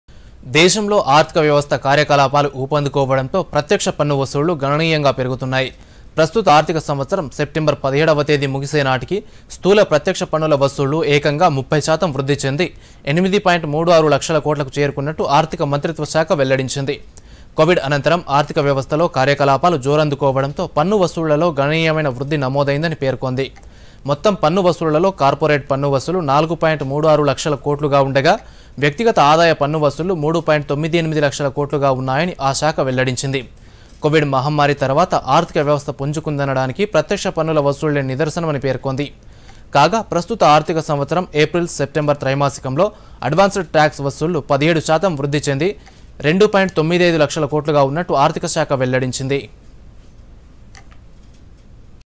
Direct tax collections are increasing significantly due to economic activities in the country
దేశంలో ఆర్థిక వ్యవస్థ కార్యకలాపాలు ఊపందుకోవటంతో ప్రత్యక్ష పన్ను వసూళ్లు గణనీయంగా పెరుగు తున్నాయి. ప్రస్తుత ఆర్థిక సంవత్సరం.సెప్టెంబరు 17వ తేదీ ముగిసే నాటికి స్థూల ప్రత్యక్ష పన్నుల వసూళ్లు ఏకంగా 30 శాతం వృద్ధి చెంది 8.36 లక్షల కోట్లకు చేరుకున్నట్లు ఆర్థిక మంత్రిత్వ శాఖ వెల్లడించింది. కొవిడ్‌ అనంతరం ఆర్థిక వ్యవస్థలో కార్యకలాపాలు జోరందుకోవటంతో పన్ను వసూళ్లలో గణనీయమైన వృద్ధి నమోదైందని పేర్కొంది. మొత్తం పన్ను వసూళ్లలో కార్పొరేట్‌ పన్ను వసూళ్లు 4.36 లక్షల కోట్లుగా ఉండగా, వ్యక్తిగత ఆదాయ పన్ను వసూళ్లు 3.98 లక్షల కోట్లుగా ఉన్నాయని ఆ శాఖ వెల్లడించింది. కొవిడ్‌ మహమ్మారి తర్వాత ఆర్థిక వ్యవస్థ పుంజుకుందనడానికి  ప్రత్యక్ష పన్నుల వసూళ్లే నిదర్శనమని పేర్కొంది. కాగా ప్రస్తుత ఆర్థిక సంవత్సరం ఏప్రిల్‌-సెప్టెంబరు త్రైమాసికంలో అడ్వాన్స్‌ టాక్స్‌ వసూళ్లు 17 శాతం వృద్ధి చెంది రూ.2.95 లక్షల కోట్లుగా ఉన్నట్లు ఆర్థిక శాఖ వెల్లడించింది.